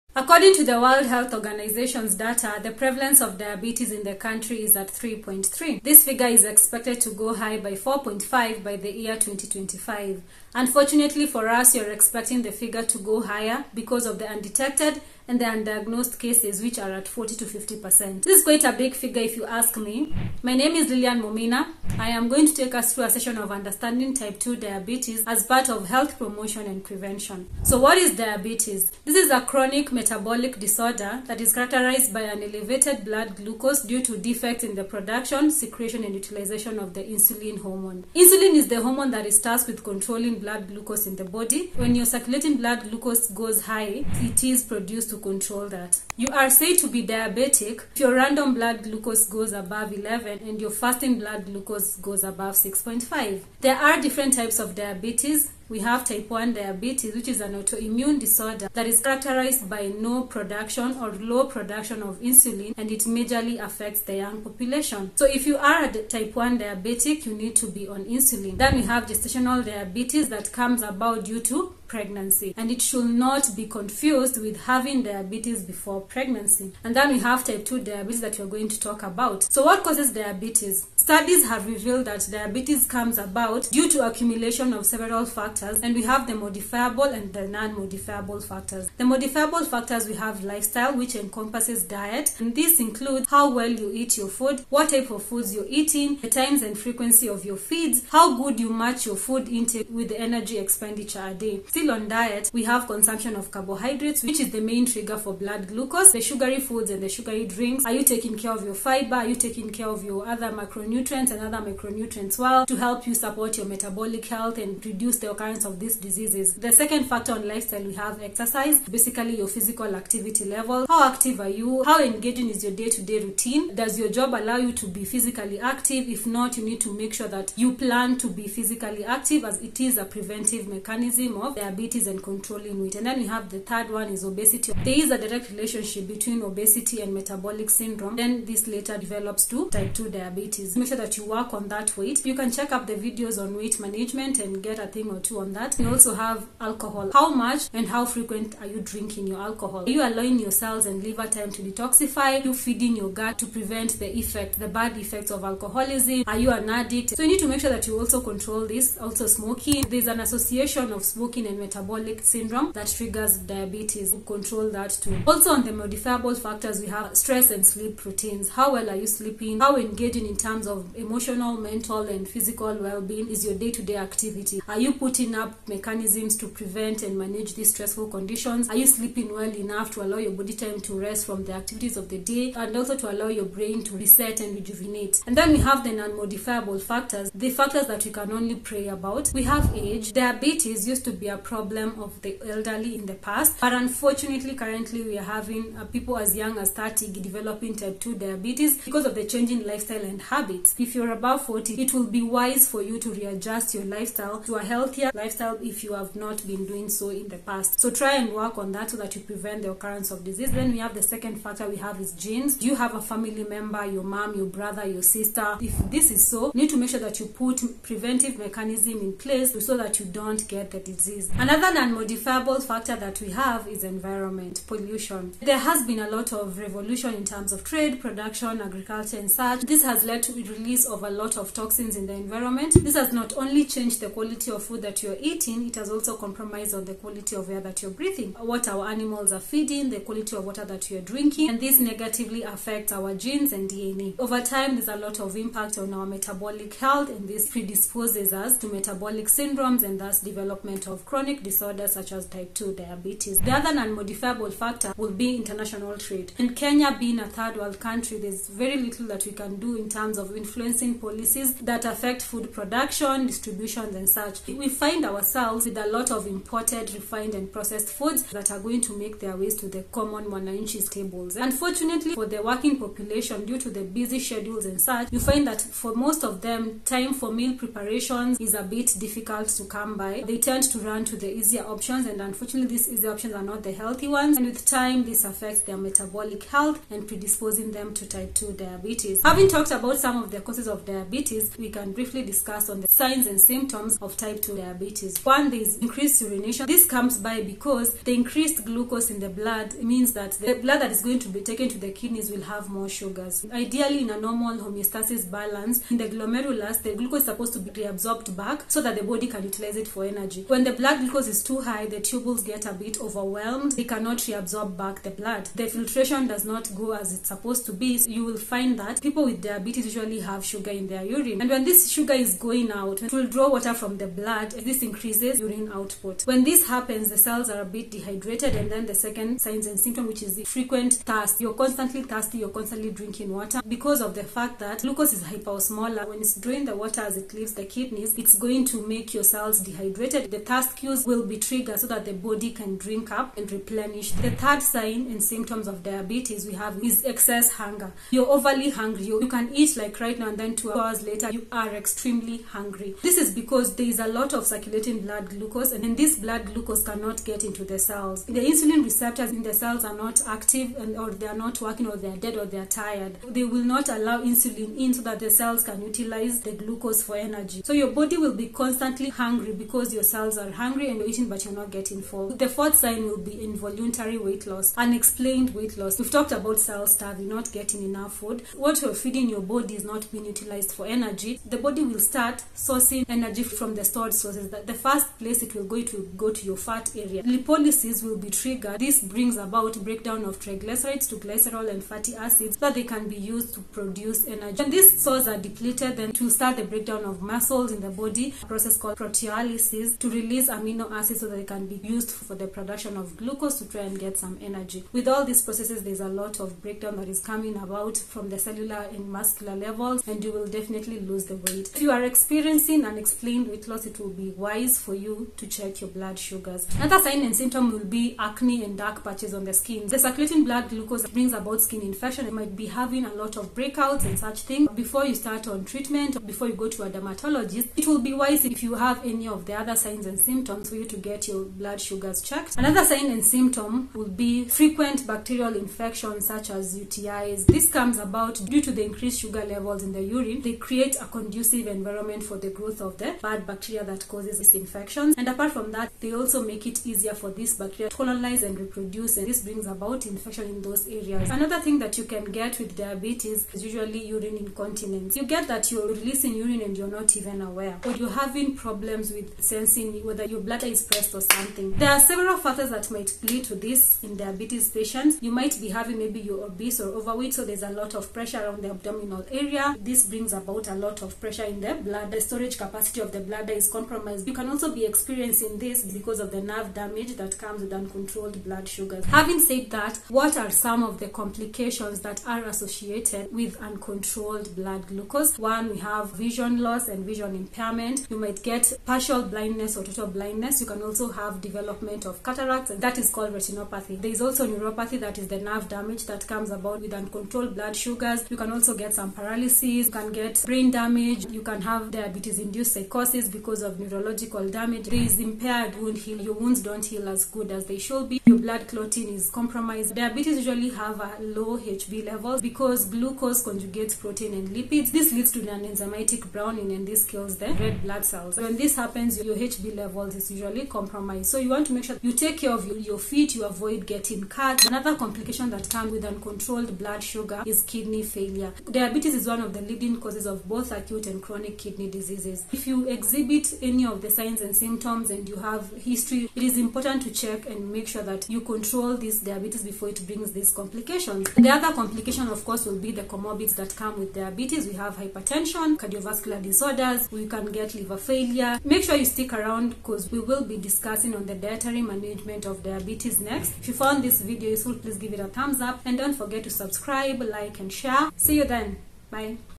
Type 2 Diabetes, Signs and symptoms and complications of Diabetes 2025
Type 2 diabetes is a chronic metabolic disorder affecting more than 40 million, in Kenya, the prevalence stands at 3.3 with projected increase as the undetected cases still remain high. Unfortunately, the primary focus in the country is on pharmacological treatment and this leaves a huge gap on education, prevention and reversal of this chronic disease. I have compiled the signs and symptoms and complications associated with uncontrolled blood sugars.lets learn and grow together.

How to loose the weight for good and reasons for the weight gain after loosing it.